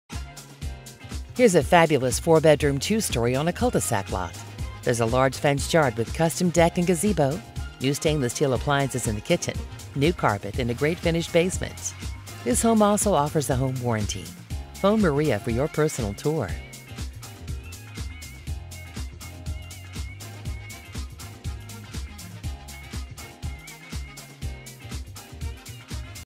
565 Blue Spruce Pl Trenton, OH | MLS# 1563833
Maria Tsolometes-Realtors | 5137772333

Fabulous 4 bedroom 2 story on cul de sac lot. Large fenced yard with custom deck/gazebo. New stainless steel appliances. New carpet. Great finished basement. Home Warranty.